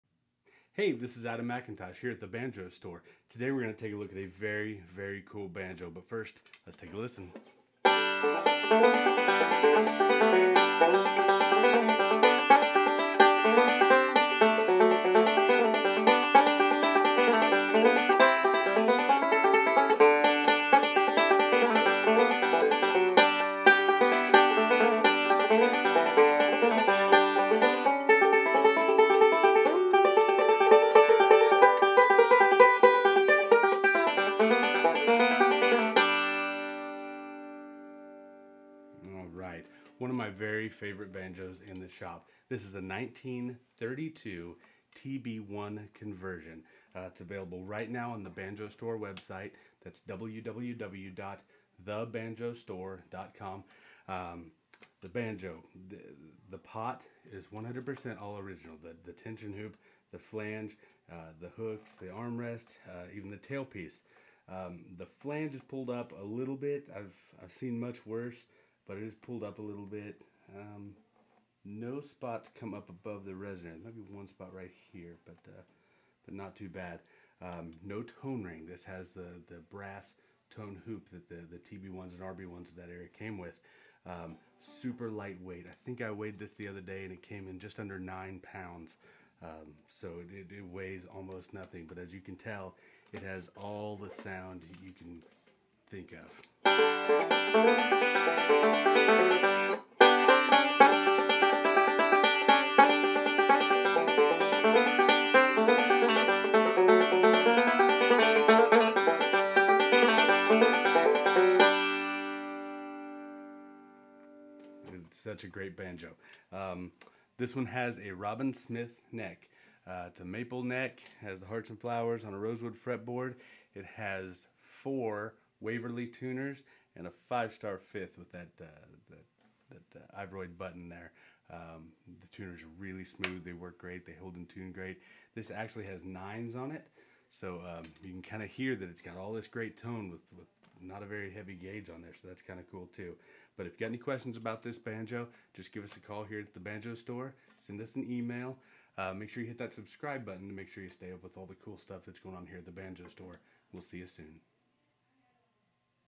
1932 TB-1 Conversion at The Banjo Store
The Banjo Store demo of a 1932 TB-1 with a maple Robin Smith neck!